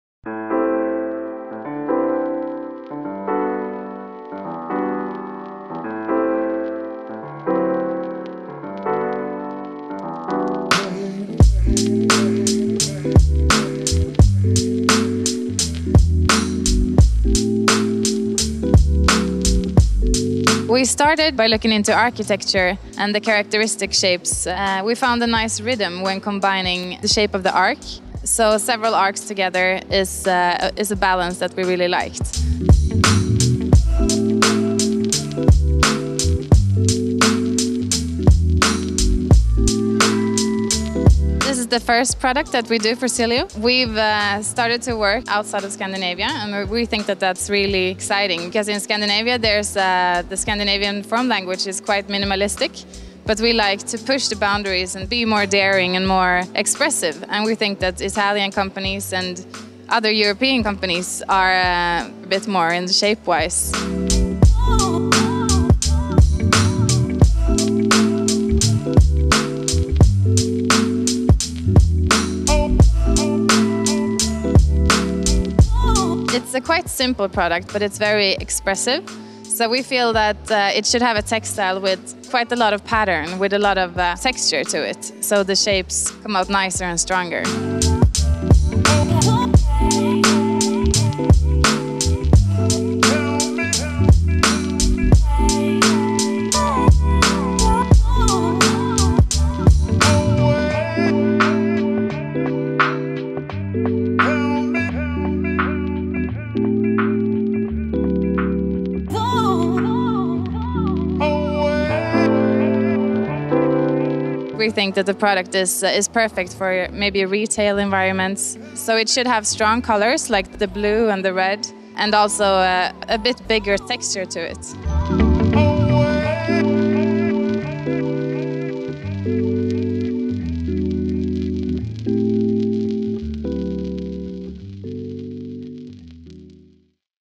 Salone del Mobile.Milano 2018 | ZILIO ALDO A&C - Malin Engvall presents Arkad the Pouf
Salone del Mobile.Milano 2018 | ZILIO ALDO A&C - Malin Engvall presents Arkad the Pouf by Zilio Aldo A&C. Interview by Archiproducts during the Salone del Mobile 2018 in Milano.